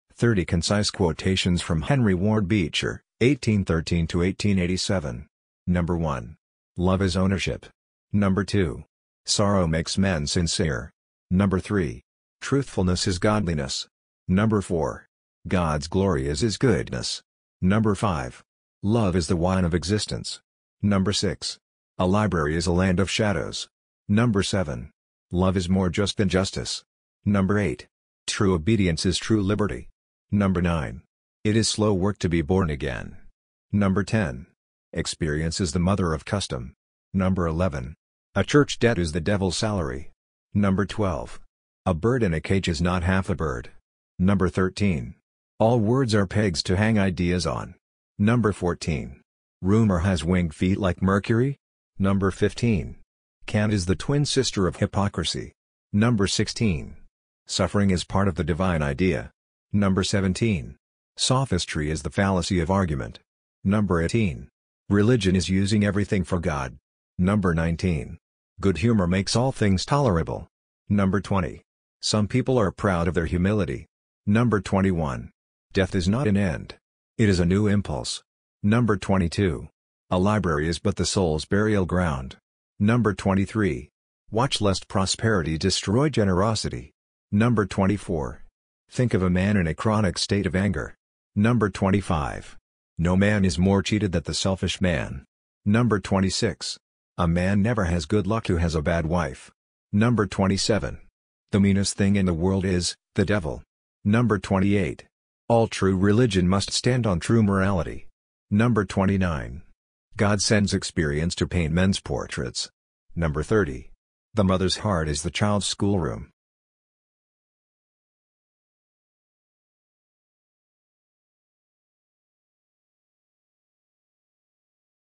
30 Concise Audio Quotes: Henry Ward Beecher (1813-1887)
Audio Quotes Listening of 30 Concise Quotations from Henry Ward Beecher (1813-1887):
#1.  Love is ownership.
#2.  Sorrow makes men sincere.
#3.  Truthfulness is godliness.
#4.  God’s glory is His goodness.
#5.  Love is the wine of existence.
#6.  A library is a land of shadows.
#7.  Love is more just than justice.
#8.  True obedience is true liberty.
#9.  It is slow work to be born again.
#10.  Experience is the mother of custom.
#11.  A church debt is the devil’s salary.
#12.  A bird in a cage is not half a bird.
#13.  All words are pegs to hang ideas on.
#14.  Rumor has winged feet like Mercury.
#15.  Cant is the twin sister of hypocrisy.
#16.  Suffering is part of the divine idea.
#17.  Sophistry is the fallacy of argument.
#18.  Religion is using everything for God.
#19.  Good-humor makes all things tolerable.
#20.  Some people are proud of their humility.
#21.  Death is not an end. It is a new impulse.
#22.  A library is but the soul’s burial ground.
#23.  Watch lest prosperity destroy generosity.
#24.  Think of a man in a chronic state of anger!
#25.  No man is more cheated that the selfish man.
#26.  A man never has good luck who has a bad wife.
#27.  The meanest thing in the world is—the devil.
#28.  All true religion must stand on true morality.
#29.  God sends experience to paint men’s portraits.
#30.  The mother’s heart is the child’s schoolroom.